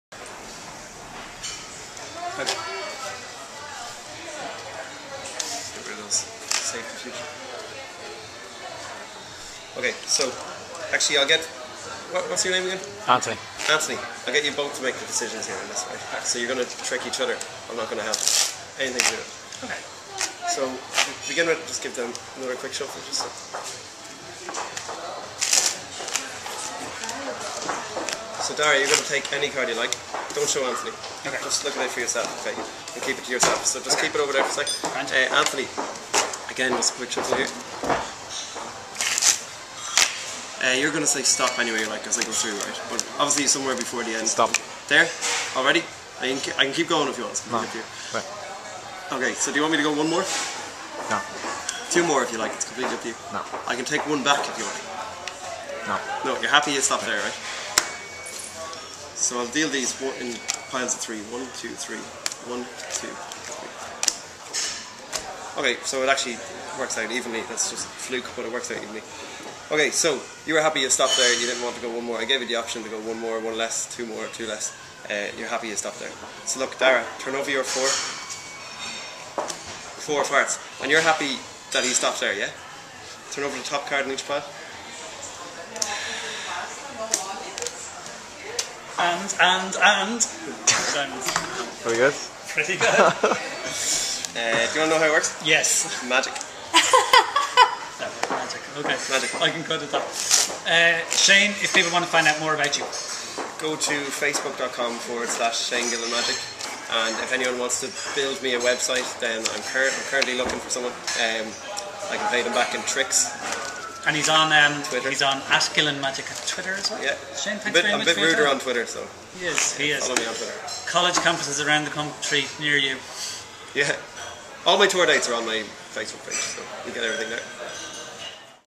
Yay.ie - Shane Gillen and some card magic
Shane Gillen performs magic as part of a interview.